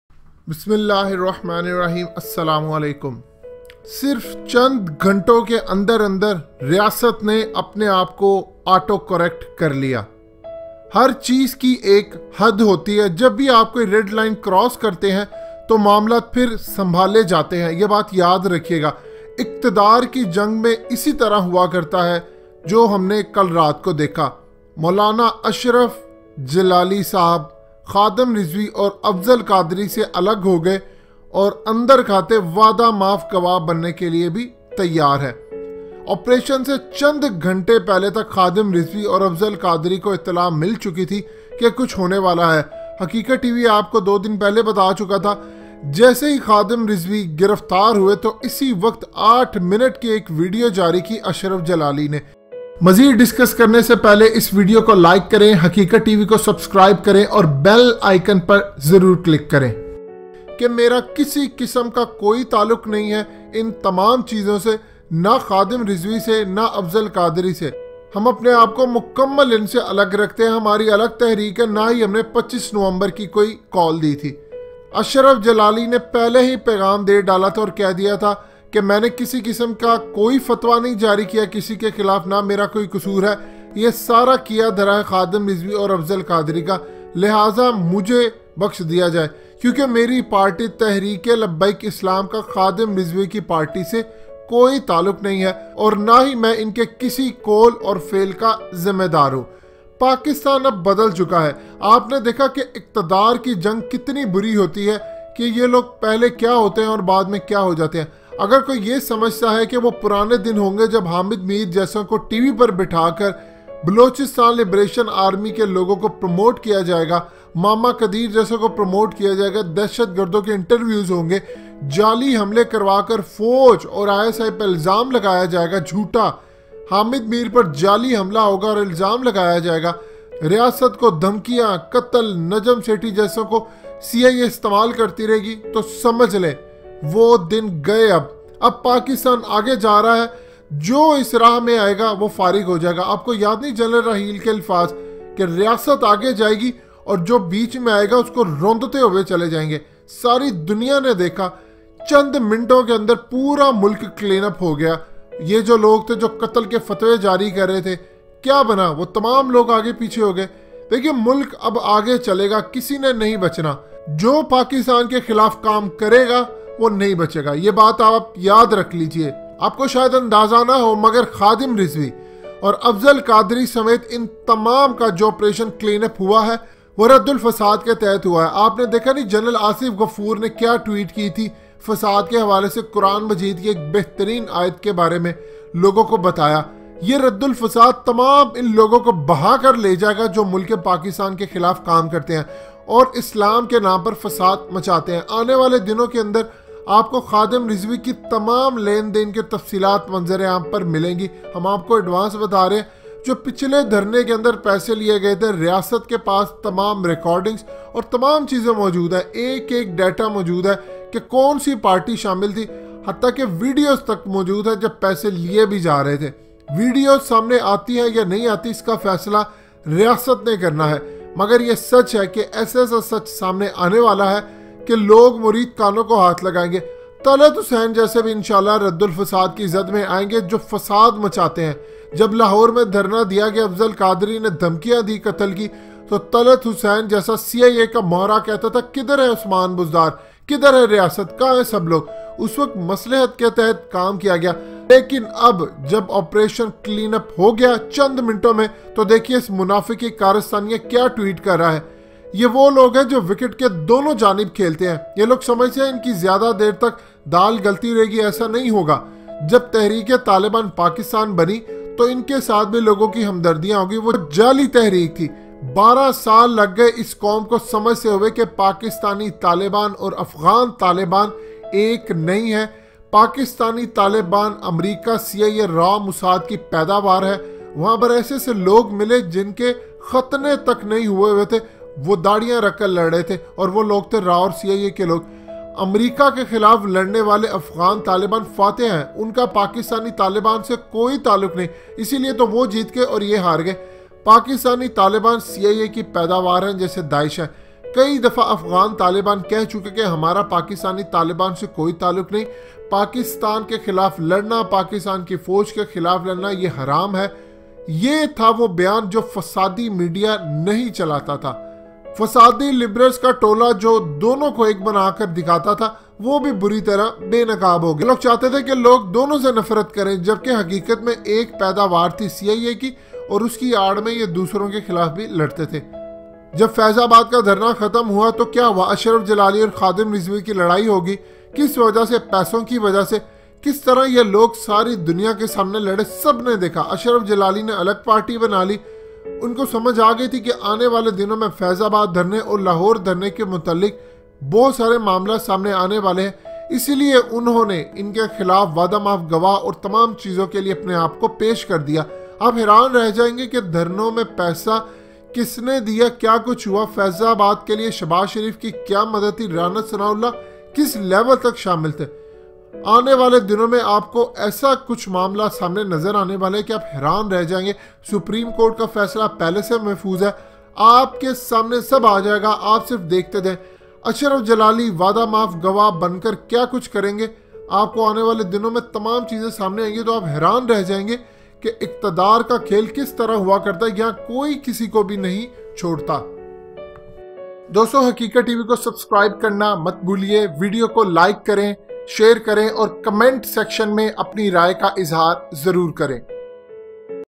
Ashraf Jalali is No More a Part of Labaik and Others in Recent Move
Ashraf Jalali has given a special message to his followers and told them not to be the part of Labaik and distanced himself as well. He is now working as a good person and not supporting Labaik anymore